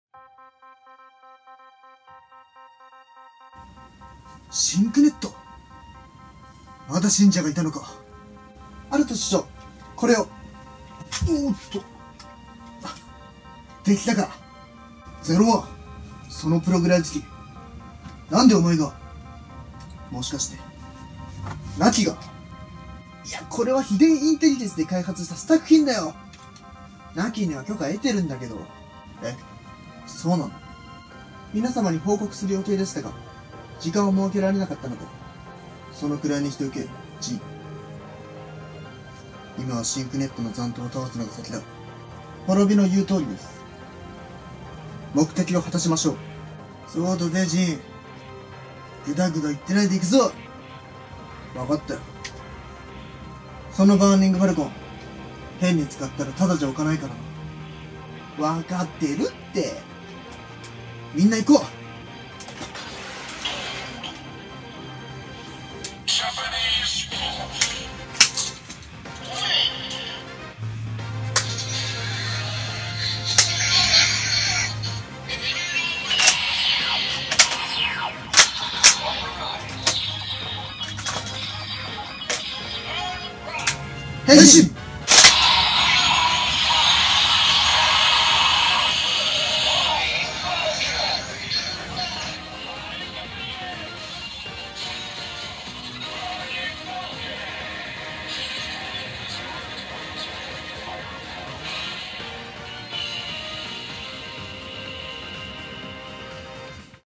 仮面ライダーゼロツーイズver&仮面ライダーゼロワンバーニングファルコン&仮面ライダー迅フライングファルコン&仮面ライダー滅アークスコーピオン&仮面ライダー雷&仮面ライダー亡
【オリジナルストーリー】
あらすじ:真紅ネットの残党を倒すため、仮面ライダーゼロワンこと飛電或人がバーニングファルコンのプログライズキーを使い、イズと滅亡迅雷の4人と一緒に戦う。
＊劇場版仮面ライダーゼロワン後、ゼロワンOthers前